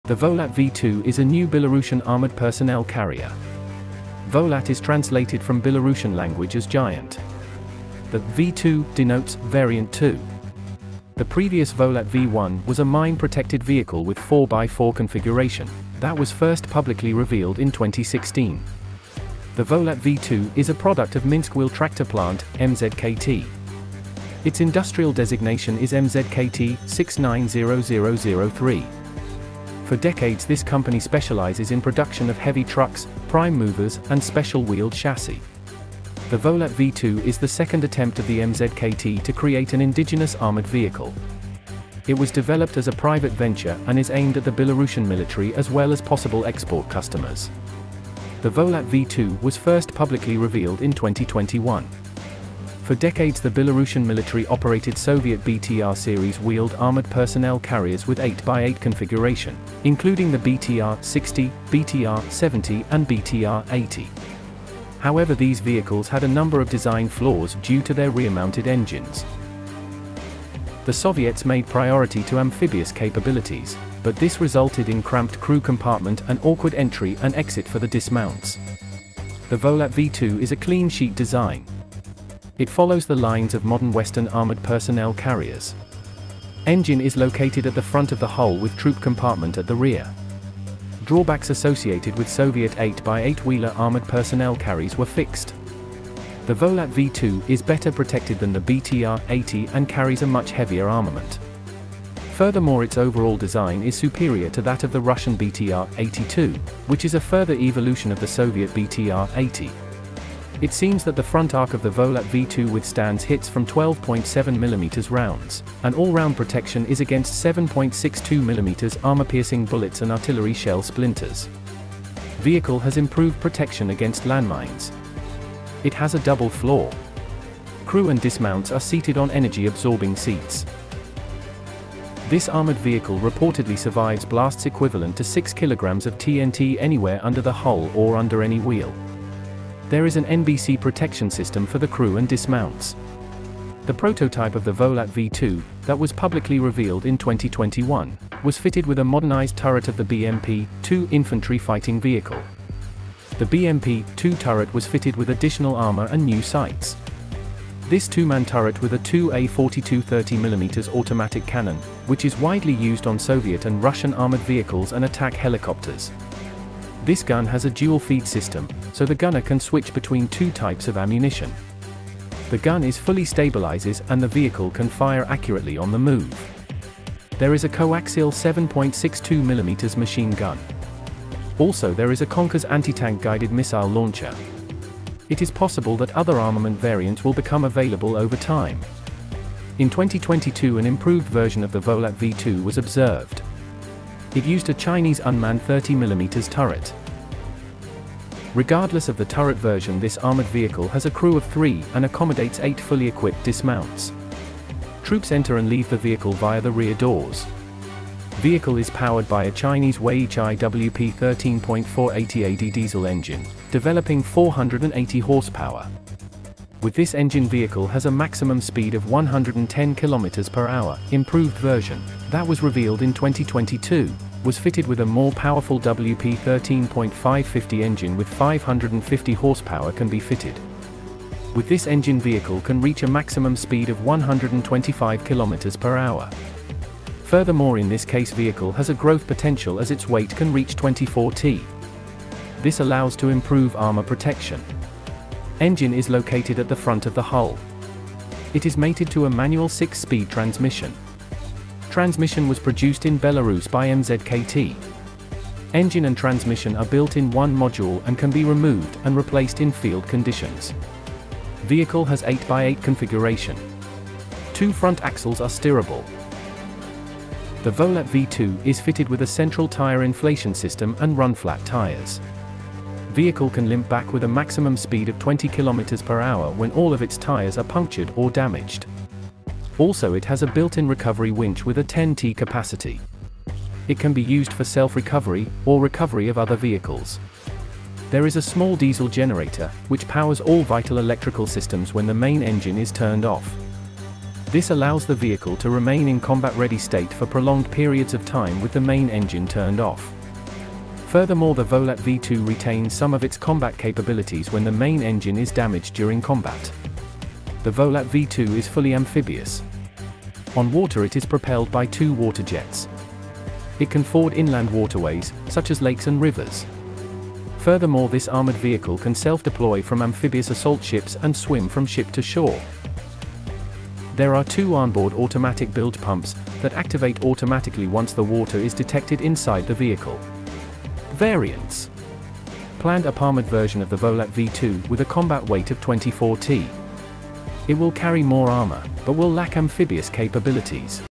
Volat V2 Armored personnel carrier
The Volat V2 is a new Belarusian armored personnel carrier. Volat is translated from Belarusian language as Giant. The "V2" denotes "Variant 2". The previous Volat V1 was a mine protected vehicle with 4x4 configuration, that was first publicly revealed in 2016. The Volat V2 is a product of Minsk Wheel Tractor Plant (MZKT). Its industrial designation is MZKT-690003. For decades this company specializes in production of heavy trucks, prime movers and special wheeled chassis. The Volat V2 is the second attempt of the MZKT to create an indigenous armored vehicle. It was developed as a private venture and is aimed at the Belarusian military as well as possible export customers. The Volat V2 was first publicly revealed in 2021.